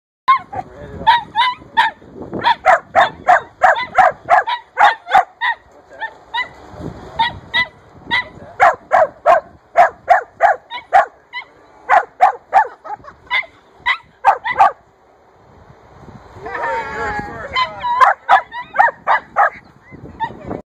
Vader's World Lure course #1 10-17-14
Vader tackles a lure course in Morgan Hill.